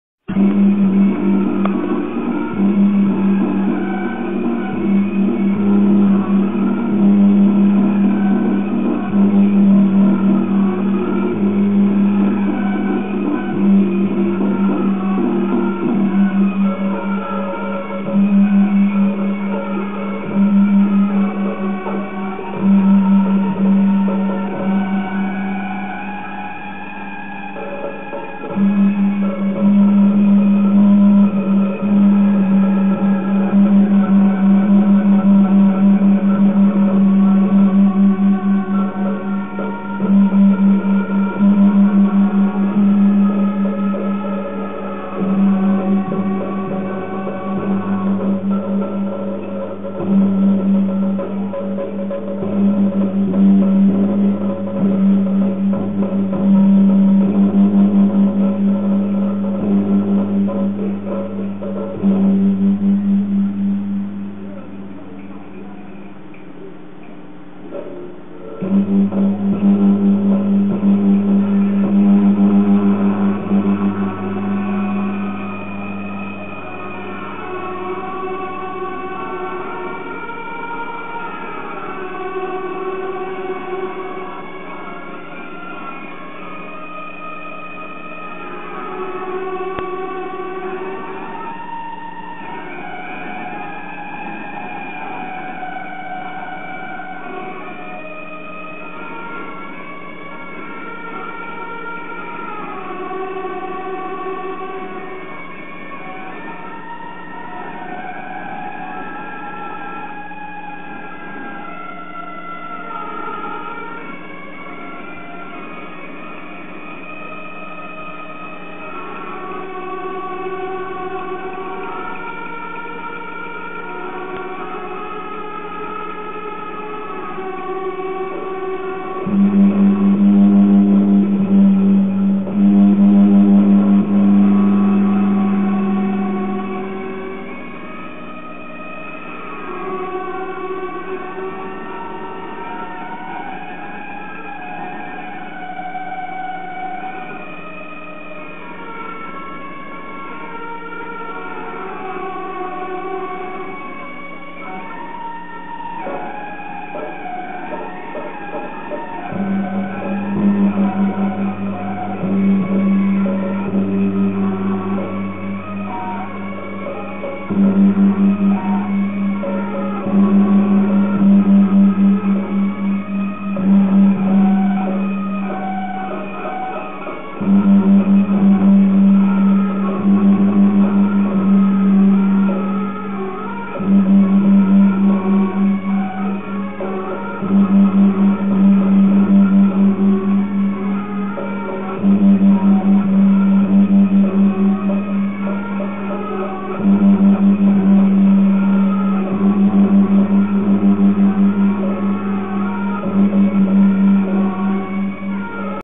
20121115南鯤鯓代天府凌霄寶殿入火安座大典之日的上午團拜紀錄 含桃園和樂軒古漢樂演奏 南鯤鯓代天府凌霄寶殿入火安座大典後的團拜紀錄 AC 480
含台灣寺廟影像檔案分享的網址:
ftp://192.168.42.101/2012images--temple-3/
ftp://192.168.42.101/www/